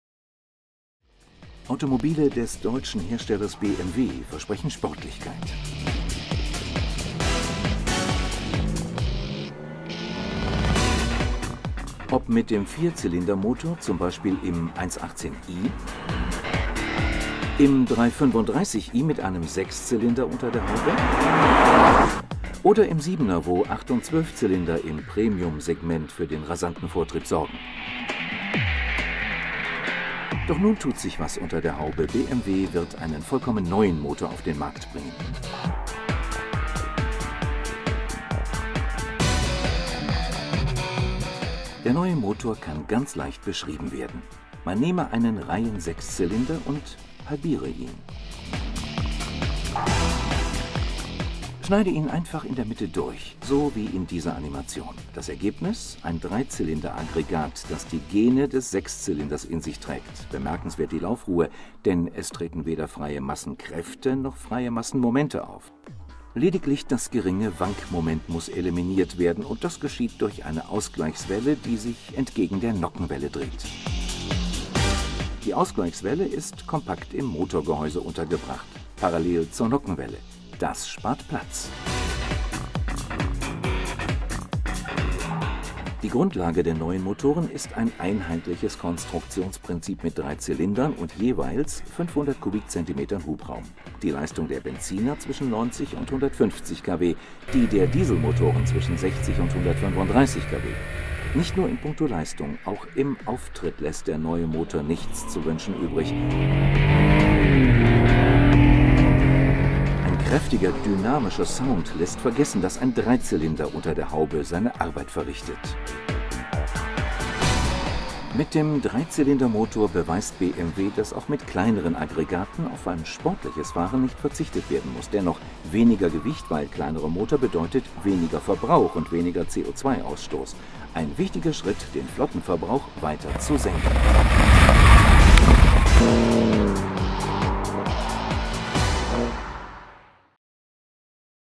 BMW 3-Zylinder-Motoren
Automobile des deutschen Herstellers BMW versprechen Sportlichkeit. Ob mit dem 4-Zylinder-Motor zum Beispiel im 118i, im 335i mit einem Sechszylinder unter der Haube oder im 7er, wo Acht- und Zwölf-Zylinder im Premium-Segment für den rasanten Vortrieb sorgen.